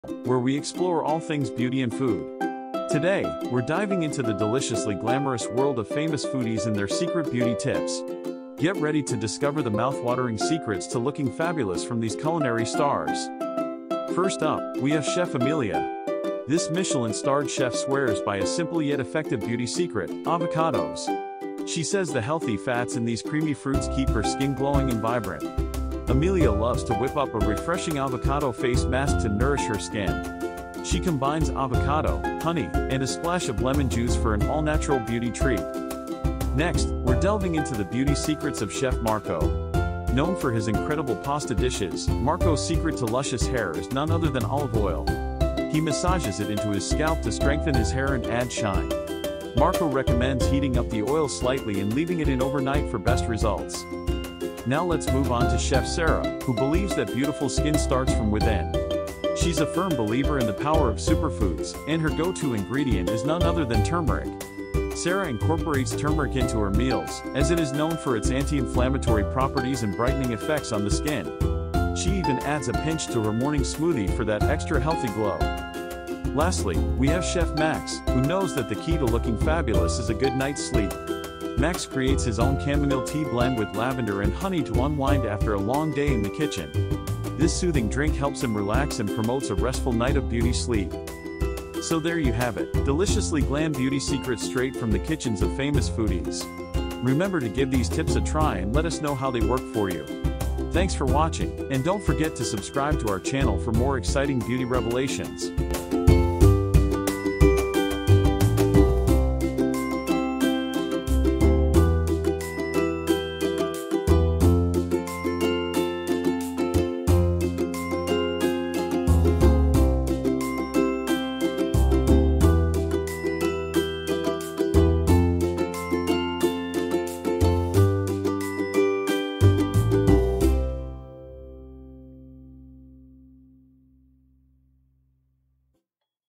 Deliciously Glam: Beauty Secrets of Famous Foodies
In this enticing episode of "Deliciously Glam: Beauty Secrets of Famous Foodies," we uncover the hidden world of beauty rituals practiced by some of the most well-known food enthusiasts. Prepare to be tantalized as we delve into the intriguing nexus of food and beauty! Join us as we venture behind the scenes with renowned chefs, popular food bloggers, and culinary influencers who have perfected the art of looking fabulous. From top-notch skincare routines to ingenious tricks for maintaining healthy hair amidst hectic kitchen adventures, this captivating video offers a sneak peek into the beauty regimens of food-loving celebrities. Indulge your senses as we explore the delectable delights these famous foodies create in their kitchens, all while balancing their commitment to self-care and beauty. Discover the lip-smacking recipes that not only tantalize your taste buds but also nourish your body, leaving you feeling and looking amazing. With insightful interviews and insider tips, "Deliciously Glam" takes you on a mouthwatering journey of self-expression and culinary ingenuity. Immerse yourself in the awe-inspiring world of these food aficionados, where beauty and gastronomy intertwine into a harmony like no other. Tune in now to witness the Beauty Secrets of Famous Foodies unfold before your eyes. Prepare to be inspired, captivated, and tempted in this extraordinary collaboration of taste, aesthetics, and self-care.

14 DIY Homemade shampoo recipes | Hair fall, Dry scalp, Head lice

Source of stock photos and videos by Canva.com